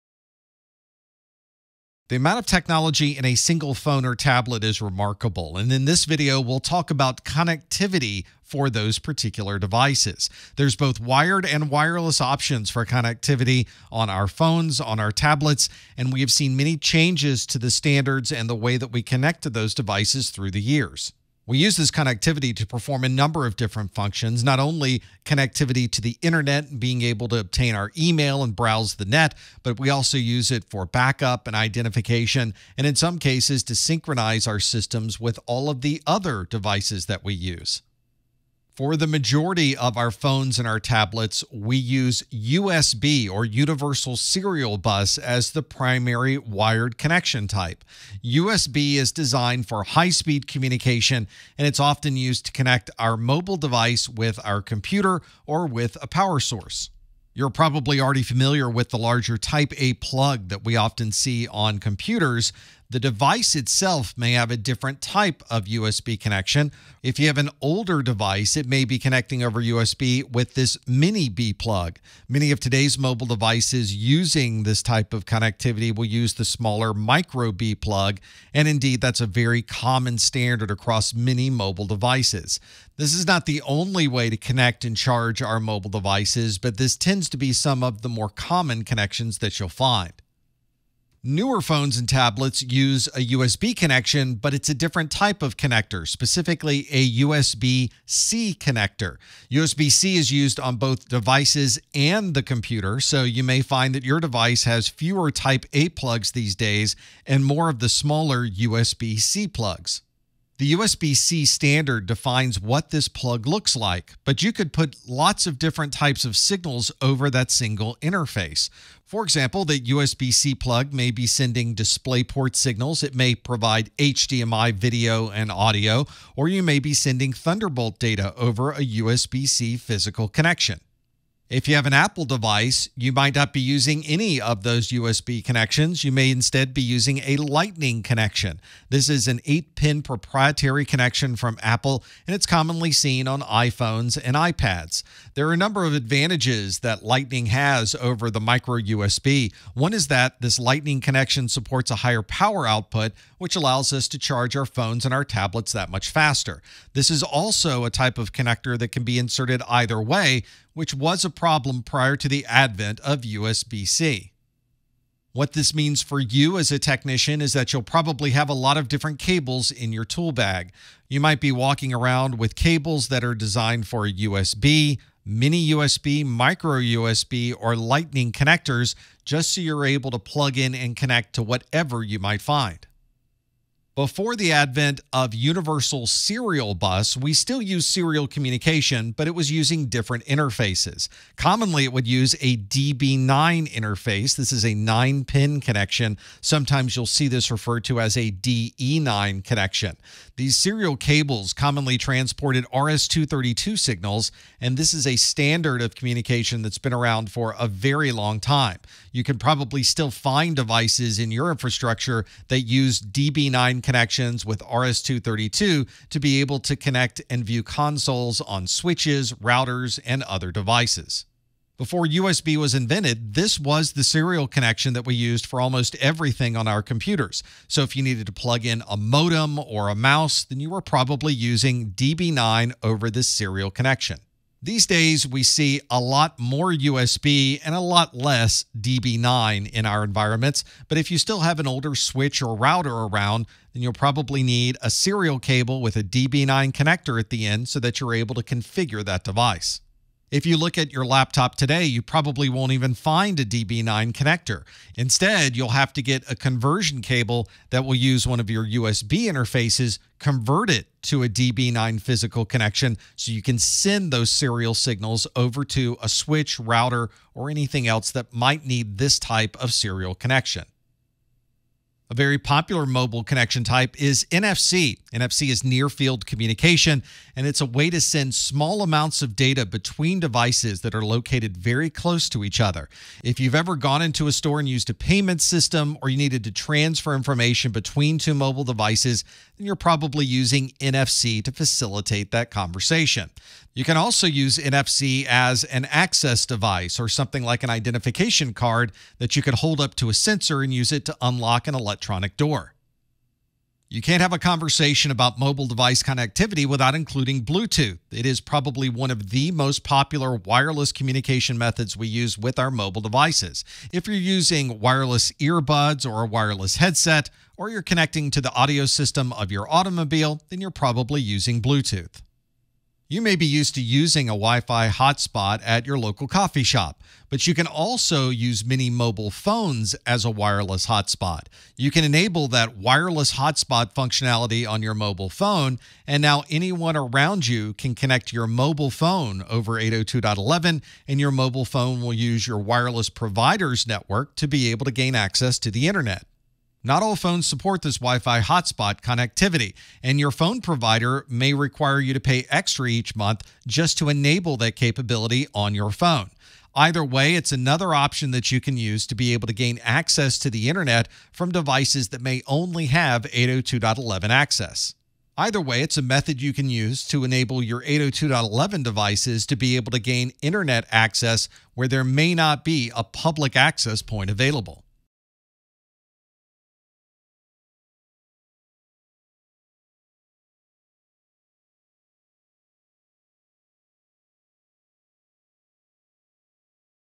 Connecting Mobile Devices - CompTIA A+ 220-1101 - 1.3
There are many ways to connect a mobile device with computers, headphones, and speakers. In this video, you’ll learn about USB connections, serial interfaces, NFC, Bluetooth, and more.